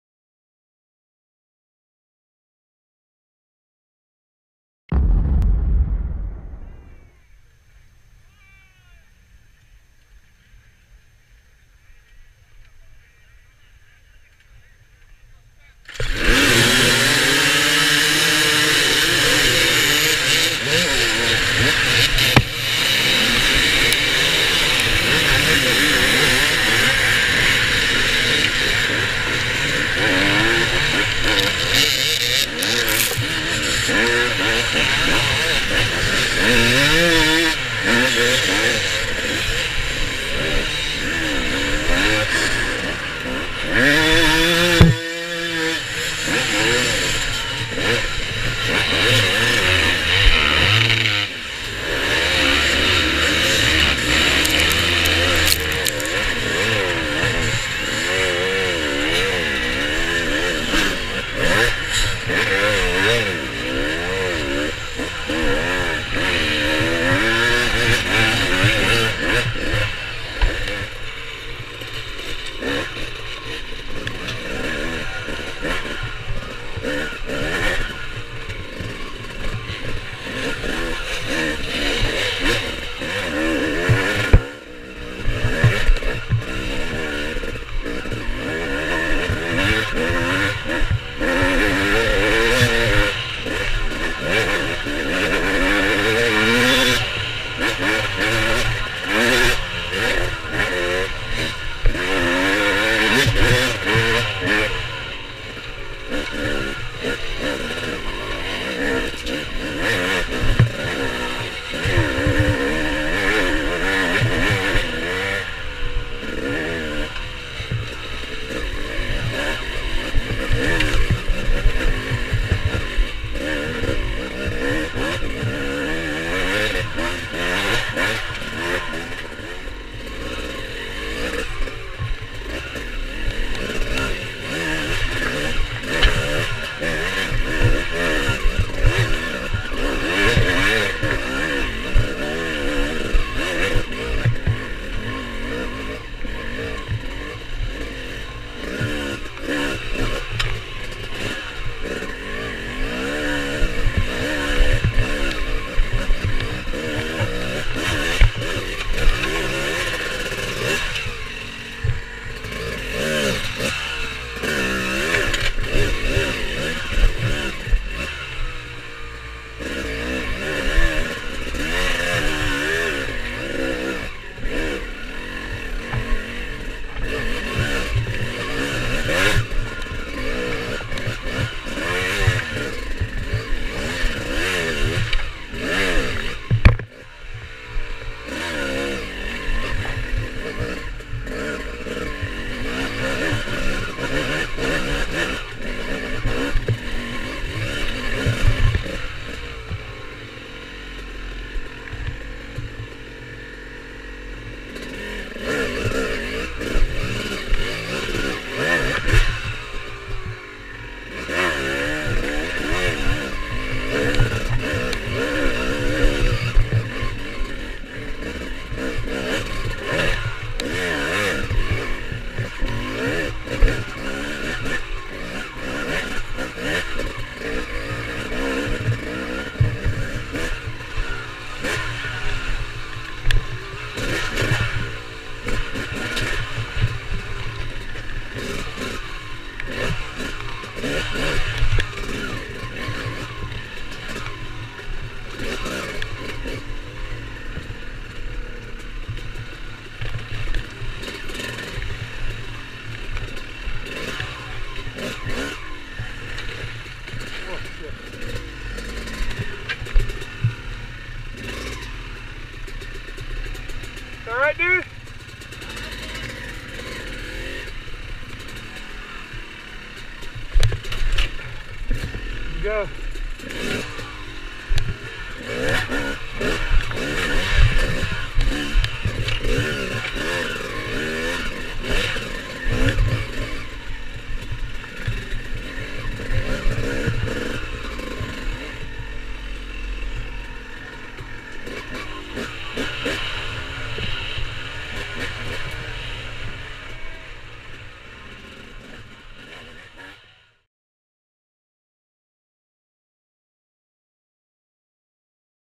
GoPro HD: 2017 King of the Motos - Justin Morgan
Hop on bord Justin Morgan's GoPro and watch highlights (and some carnage) from the night race at the 2017 King if the Motos Hard Enduro.